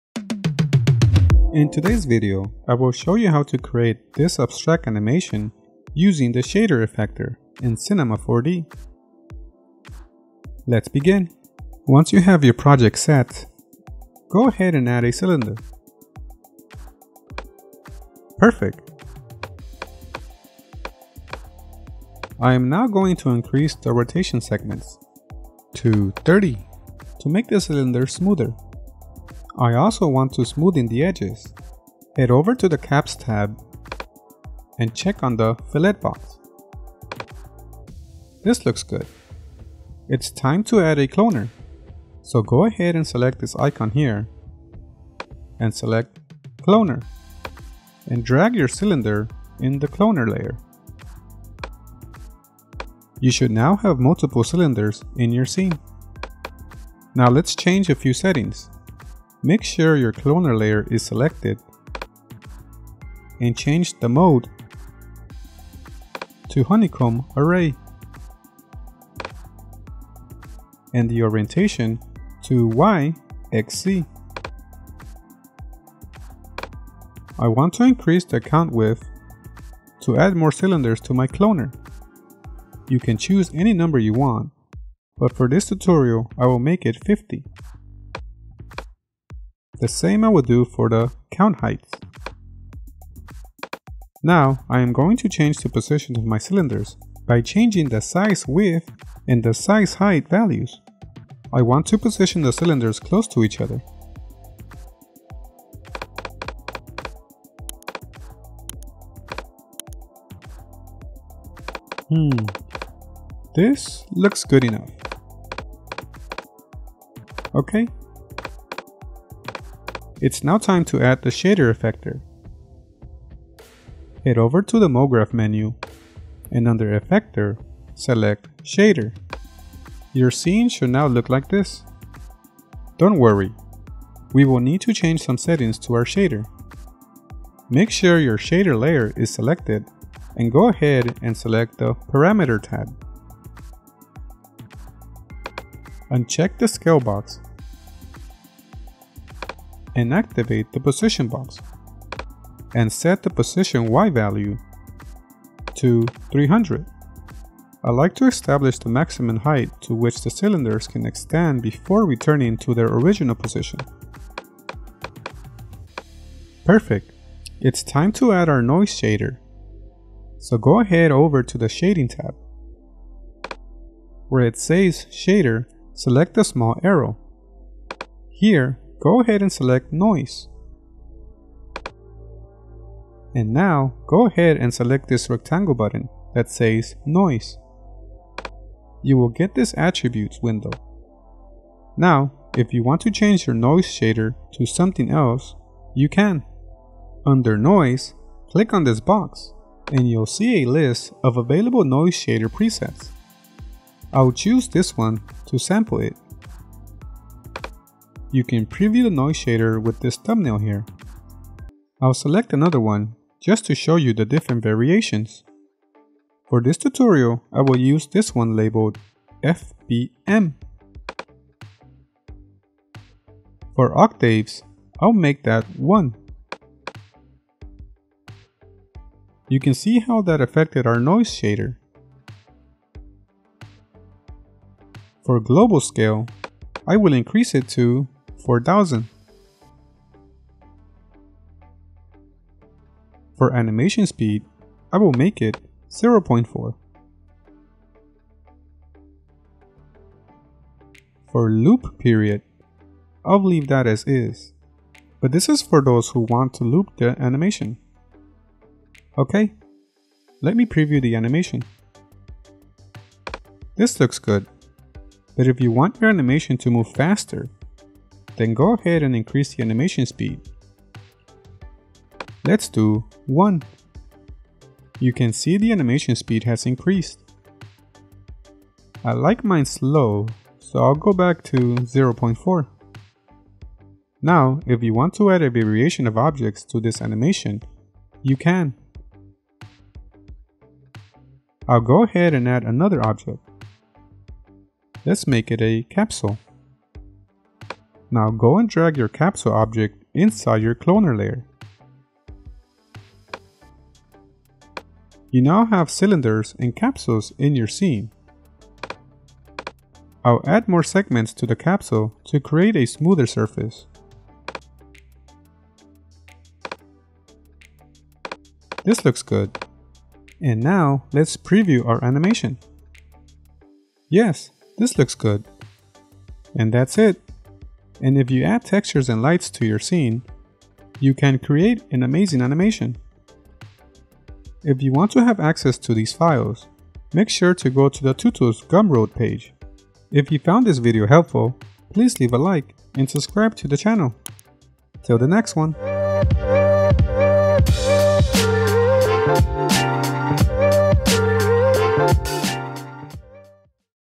Make Your First 3D Animation in Cinema 4D Under 5 Minutes! - Tutorial
Master abstract patterns and animation in Cinema 4D! Learn Emitters and Shader Effectors.

00:00 - Intro
00:14 - Add Cylinder object
00:40 - Add Cloner tool
02:02 - Add Shader Effector
02:49 - Add Shader Noise
03:36 - Select FBM Shader Noise Preset
04:44 - Add Capsule object to Cloner